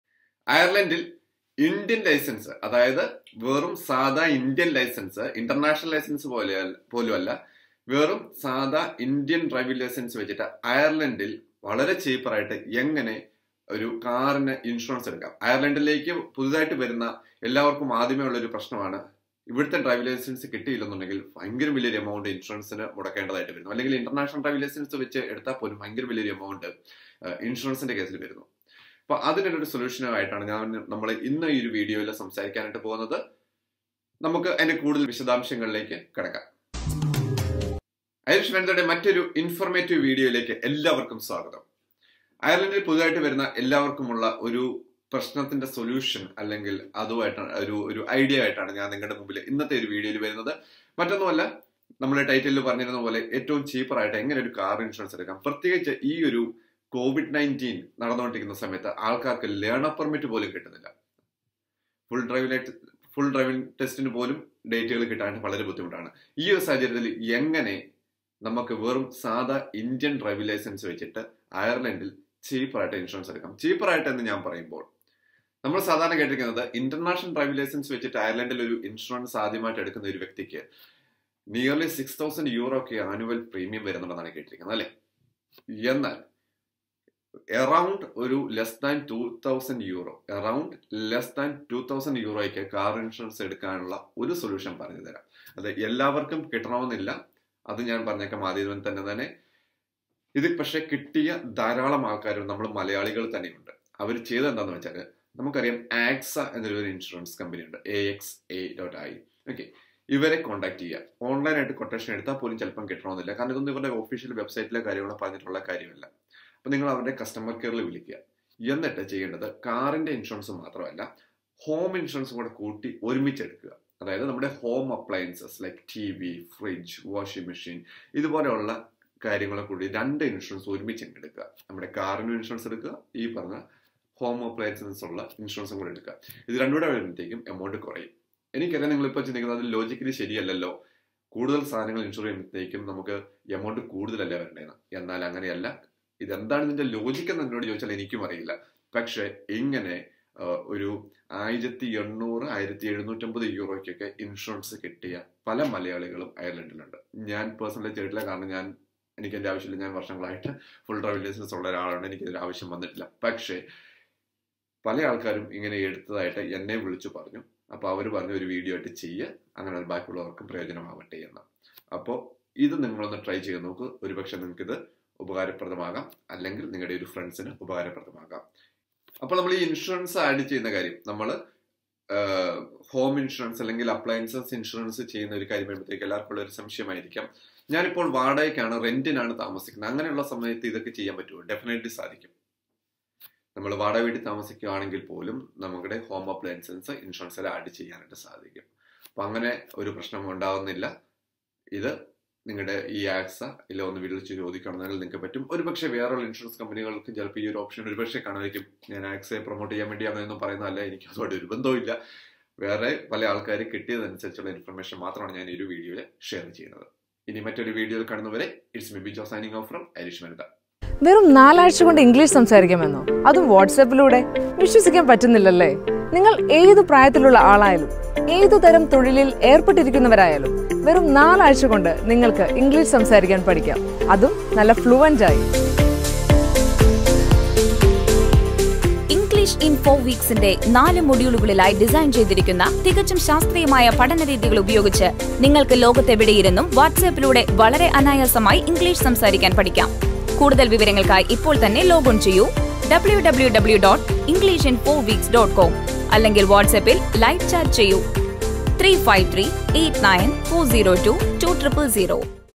How to get car insurance with Indian Driving licence in Ireland
How to get car insurance with Indian Driving licence in Ireland. അയർലണ്ടിൽ നോർമൽ  ഇന്ത്യൻ ഡ്രൈവിംഗ് ലൈസൻസ് ഉപയോഗിച്ച്  കുറഞ്ഞ തുകയ്ക്ക് കാർ ഇൻഷുറൻസ് .